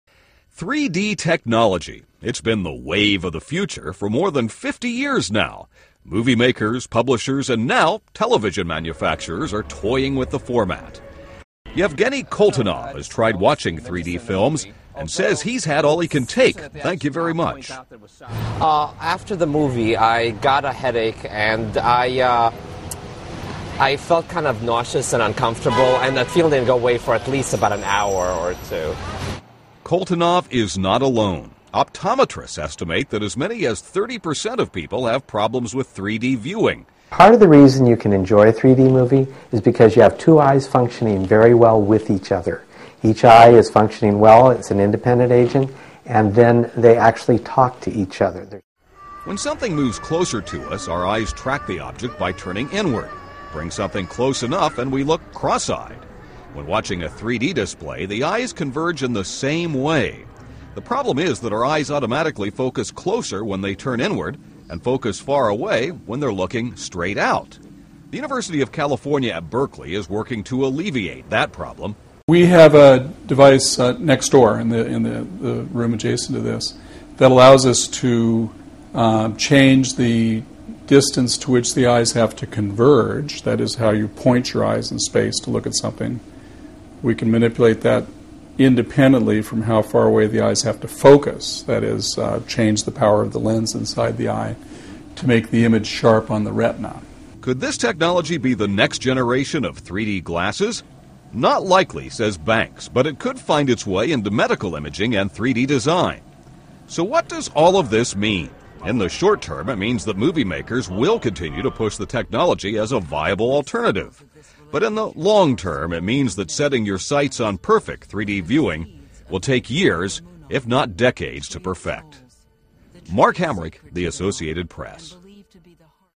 3-D Technology Continues to Struggle
3D technology has been the wave of the future for more than fifty years now. Yet despite decades of work, today's 3-D technology offers, at best, an incomplete illusion, and at worst, makes people uncomfortable or even sick. (Jan. 20)